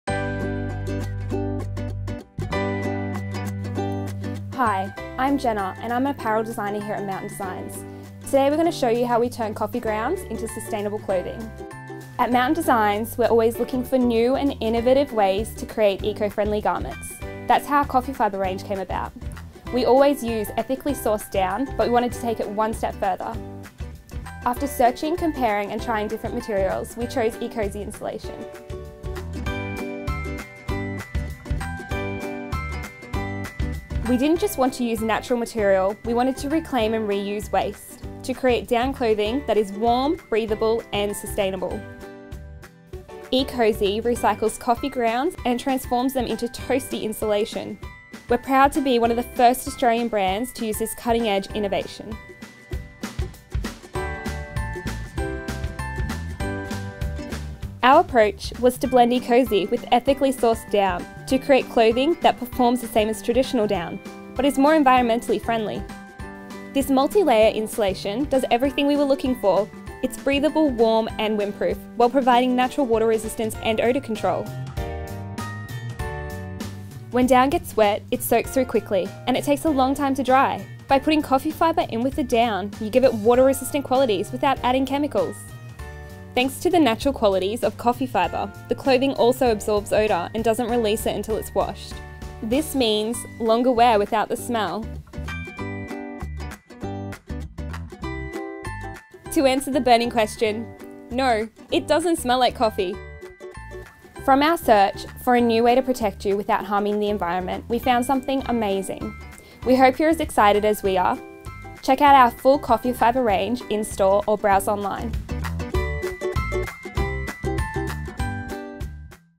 Clothing Made from coffee - turning coffee grounds into sustainable clothing
Check out how we turn coffee grounds into sustainable clothing!